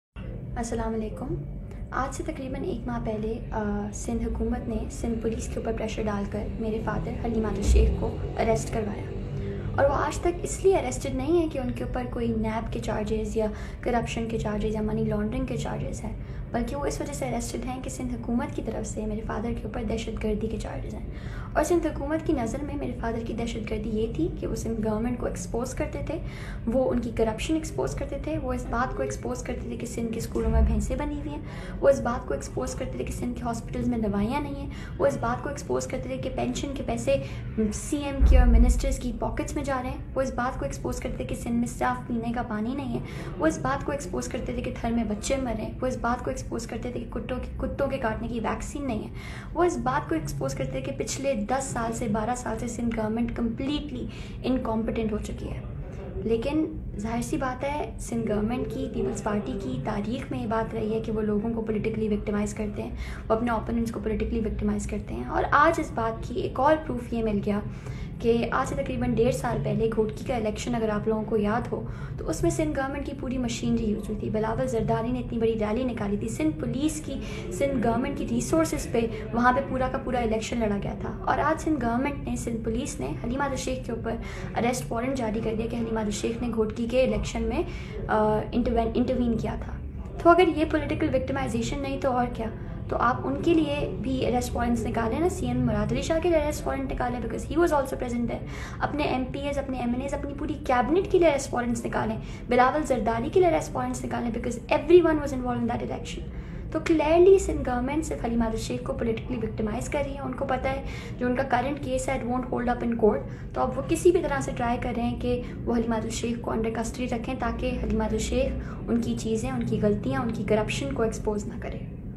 حلیم عادل شیخ کو ایک اور کیس میں گرفتار کرنے پر ان کی بیٹی عائشہ حلیم کا ویڈیو پیغام
میرے والد حلیم عادل شیخ کو پیپلزپارٹی کے پریشر میں سندھ پولیس نے گرفتار کیا ہے: عائشہ حلیم عادل
ایک ماہ سے زائد دن سے وہ اس لئے گرفتار نہیں ہیں کہ ان پر کوئی نیب یا کرپشن کیسز ہیں۔ عائشہ حلیم عادل
میرے والد سندھ حکومت کی کرپشن کو بے نقاب کرنے جرم  گرفتار ہیں۔ عائشہ حلیم
میرے والد اس لئے گرفتار ہیں وہ اسکولوں میں بھینسیں بندھی دکھاتے تھے۔ عائشہ حلیم
میرے والد اس لئے گرفتار ہیں کیوں کہ وہ اسپتالوں میں دوائیوں نہ ملنے کی شکایات کرتے تھے۔ عائشہ حلیم
میرے والد تھر کے مسائل گندے پانی کی فراہمی سمیت دیگر عوامی مسائل کو اجاگر کرتے تھے۔ عائشہ حلیم
ان کا صرف یہ قصور تھا کہ وہ ہمیشہ مظلوموں کے ساتھ کھڑے ہوتے تھے۔ عائشہ حلیم
حلیم عادل شیخ کو عوام کی حقیقی ترجمانی کرنے کی آج سزا دی جارہی ہے۔ عائشہ حلیم
ایک بار پھر سندھ حکومت نے انتقامی کارروائی کرتے ہوئے گھوٹکی الیکشن کےپرانی کیس میں گرفتار کروایا ہے۔ عائشہ حلیم
گھوٹکی الیکشن میں بلاول زرداری مراد علی شاہ سمیت دیگر وزراء نے بھی الیکشن مہم چلائی تھی۔ عائشہ حلیم
اگر حلیم عادل شیخ پر الیکشن مہم میں جانے پر ایف۔آئی آر درج کی جاتی ہے تو باقیوں پر کیوں نہیں ہوئی؟ عائشہ حلیم
گھوٹکی الیکش میں مہم چلانے پر بلاول زرداری مراد علی شاہ سمیت تمام وزراء پر بھی ایف آئی آر درج کی جاتی، عائشہ حلیم
لیکن پیپلزپارٹی نے ہمیشہ انتقامی کارروائیوں کی سیاست کی ہے۔ عائشہ حلیم
سندھ حکومت چاہتی ہے کہ ہر صورت میں حلیم عادل شیخ بند رہیں تاکہ وہ کرپشن کرتی رہے۔ عائشہ حلیم
سندھ حکومت چاہتی ہے کہ انکی کرپشن کو بے نقاب کرنے والا کوئی نہ ہو ۔ عائشہ حلیم